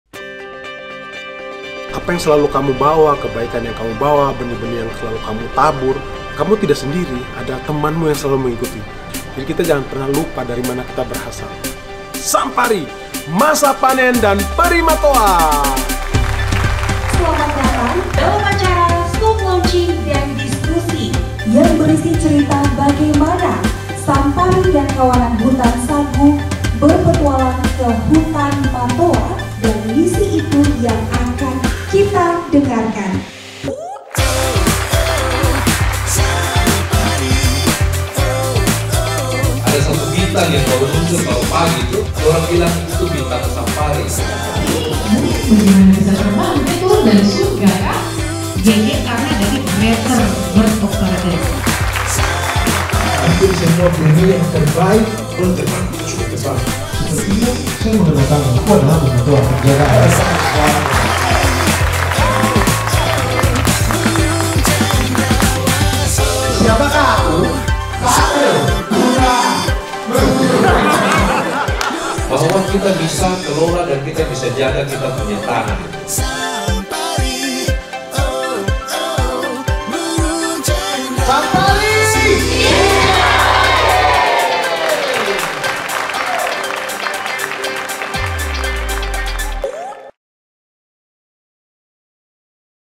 Sampari dan Peri Matoa
Halo kawan! Mari berkenalan deng Sampari si burung cenderawasih merah dan ikut berpetualang ke hutan hingga bertemu dengan Momotoa, si peri matoa. Kam bisa simak ceritanya melalui buku cerita anak Sampari: Masa Panen dan Peri Matoa!

Dapatkan buku cerita anak ini dengan menghubungi Instagram @sampari.id dan pesan bukunya cepat-cepat sebelum kehabisan!